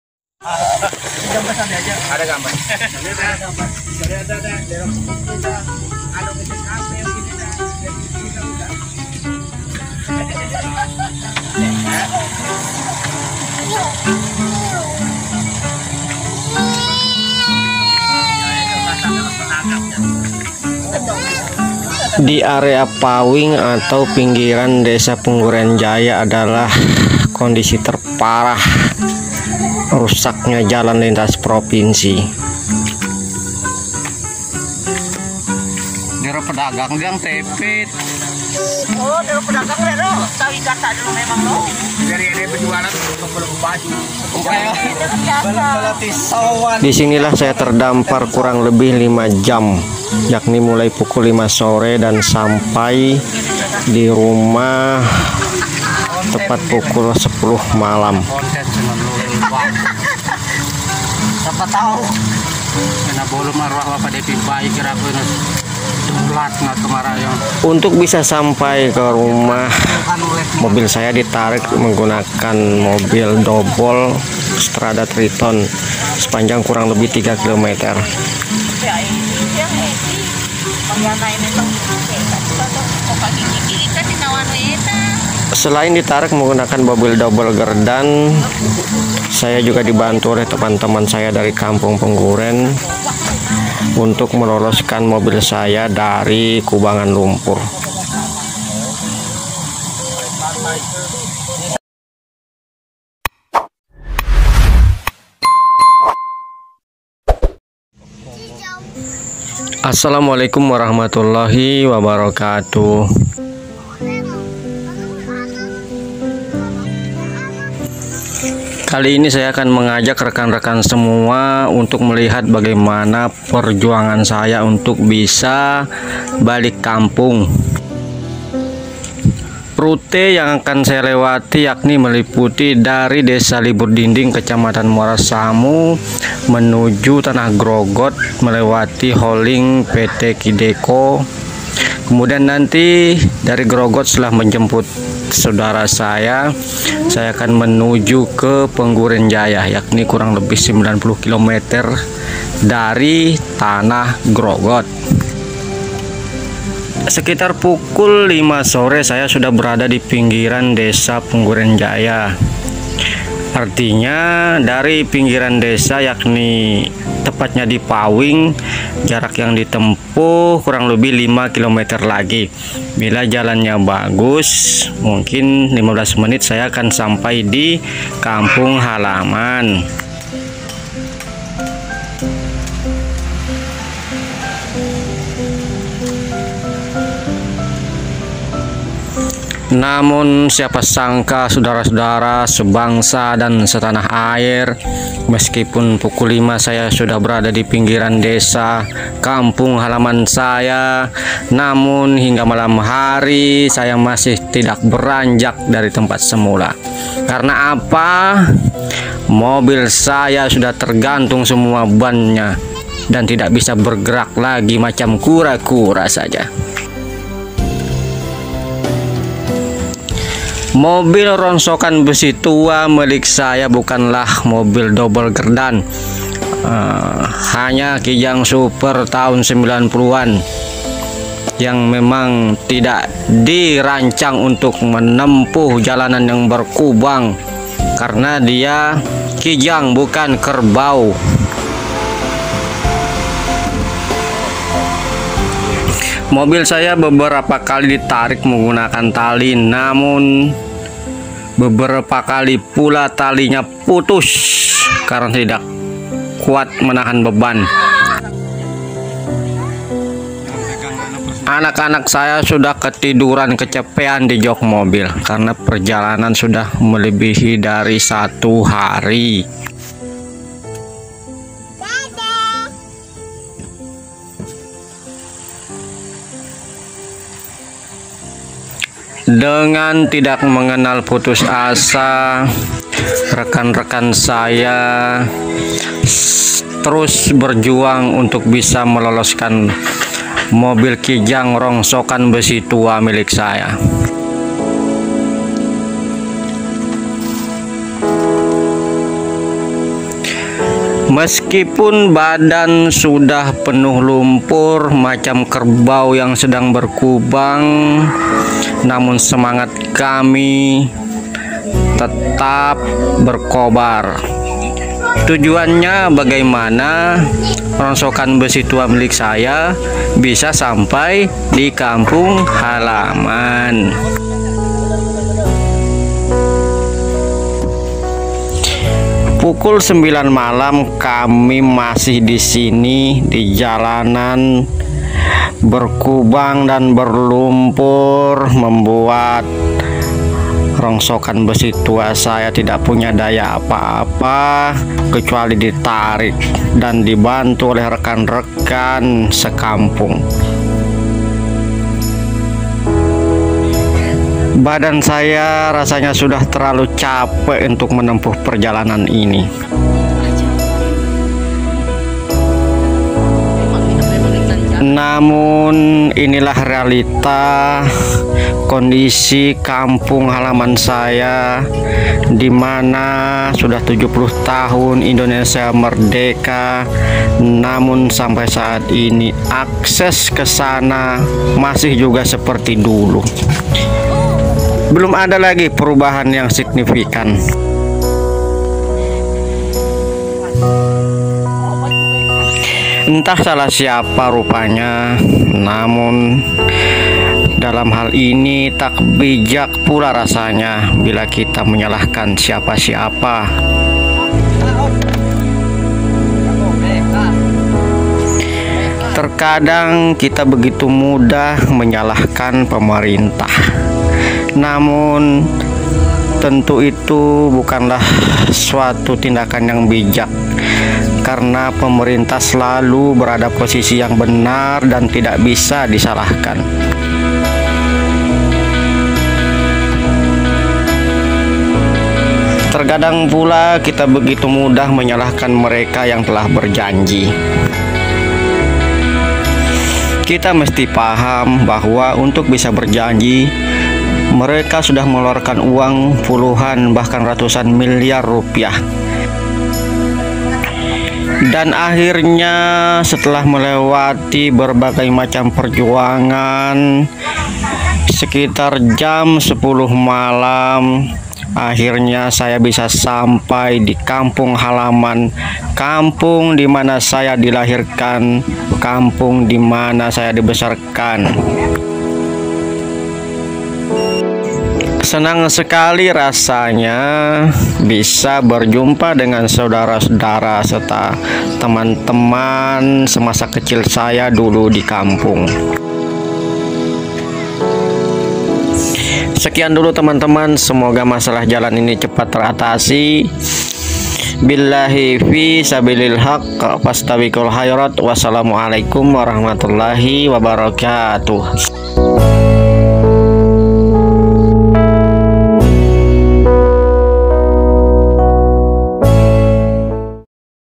Ternyata begini kondisi jalan ke kampungku...bagus banget!
kenangan mudik tahun 2022. bukan terkena razia masker  dan vaksin yang membuat perjalanan saya terhambat. tapi kondisi jalanan yang tak layak pakai